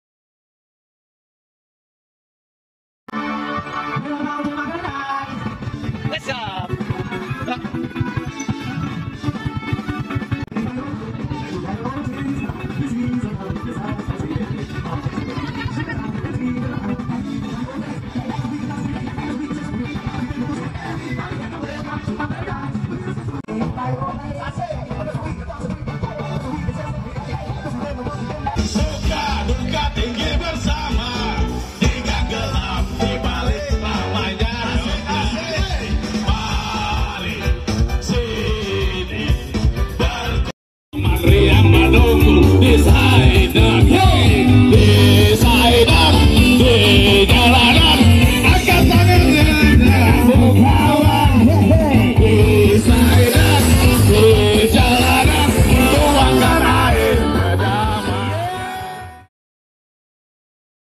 Fun Bike time
Late post :  FUN BIKE COMMUNITY Saturday, February 25th 2023
Special thanks to :
@beachpoint.anyer you have the best hangout place in Anyer 😊
@virtualband.id You guys are the best band that can liven up the atmosphere 🥰
Buat yang mau bikin acara happy2...siapin aja budgetnya, biar kami yang atur tempat n acaranya 😘